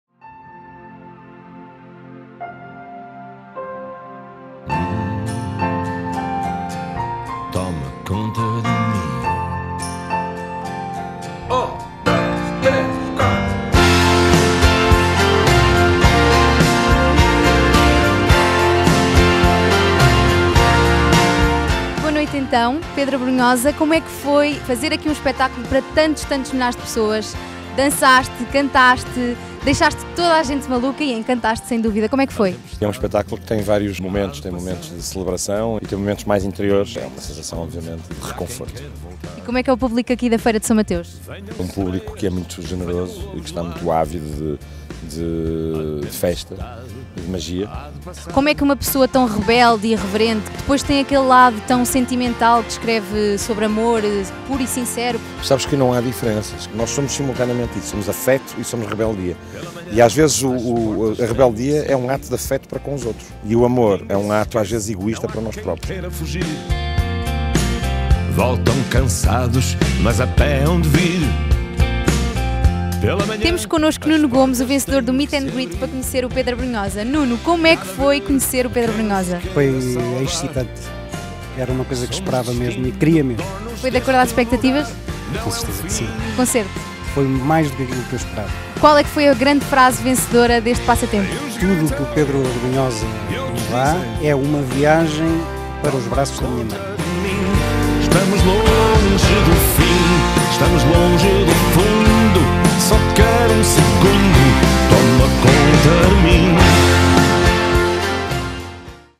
Concerto de Pedro Abrunhosa na Feira de São Mateus
O concerto de Pedro Abrunhosa na Feira de São Mateus 2015 foi a 14 de agosto. Fica aqui um breve resumo, com entrevista ao artista e ao vencedor do passatempo de Facebook para conhecer o cantor nos bastidores.
A TV da Feira está no Meo Kanal, na posição 8008.